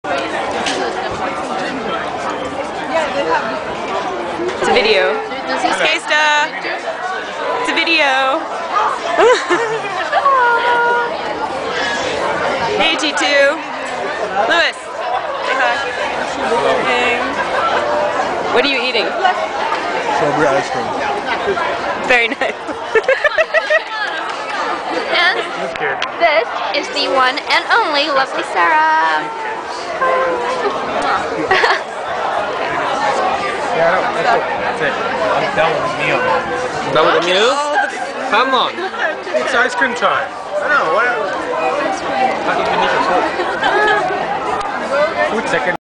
The Last BBQ
Templeton College's final bbq and last hurrah at Egrove Park, Oxford.  Just a bunch of students hanging around and eating...